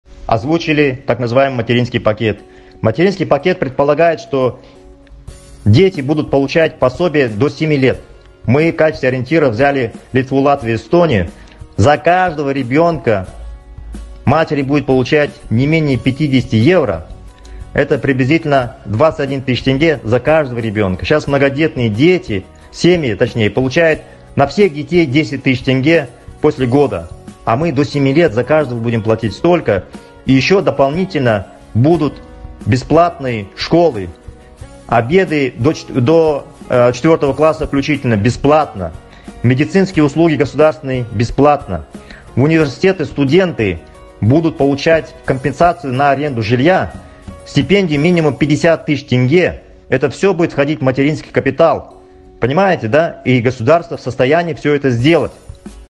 К МИТИНГУ 22 ФЕВРАЛЯ: О МАТЕРИНСКОМ КАПИТАЛЕ И ДРУГИХ РЕФОРМАХ ДВК
Мы объявили об организации митинга 22 февраля - ЗА ДОСТОЙНУЮ ЖИЗНЬ!

Многие еще не в полной мере ознакомлены с программами ДВК, которые нынешняя власть в состоянии реализовать немедленно, а не обещать хорошую жизнь в далеком будущем.
Итак, мы начинаем озвучивать ряд инициатив ДВК, которые правительство может внедрить немедленно:
1. Бесплатные обеды для детей до 4 класса включительно.
2. Выплачивать пособия за КАЖДОГО ребенка не менее 50 евро(21 тыс тенге по текущему курсу к евро). Причем выплачивать до 7(!) лет включительно!
3. Государственные медицинские услуги бесплатно.
4.  Государственное высшее и средне-специальное образование БЕСПЛАТНО.
5. Выплачивать стипендии студентам не менее 50 тыс тенге.
6. Нуждающимся в жилье студентам выплачивать компенсации на аренду жилья не менее $100 ежемесячно.

1. Загрузите анонимный мессенджер Telegram на смартфон или компьютер с официального сайта
    2. Зарегистрируйте профиль с помощью мобильного номера